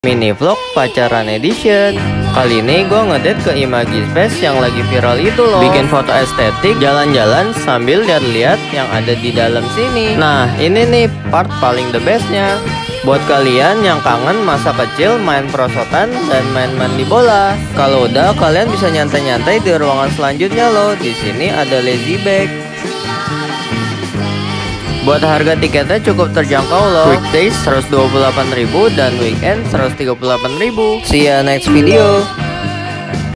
‼️ DATE IDEAS DI JAKARTA ‼️vlogger
Kali ini gw ngedate ke salah satu tempat menarik di @plaza_senayan nihh , namanya @imagi.space , tempatnya asik dan cocok buat kalian yang suka buat foto2 aesthetic lohh , yukk nonton ampe abis biar kalian tau keseruan di dalem sini ..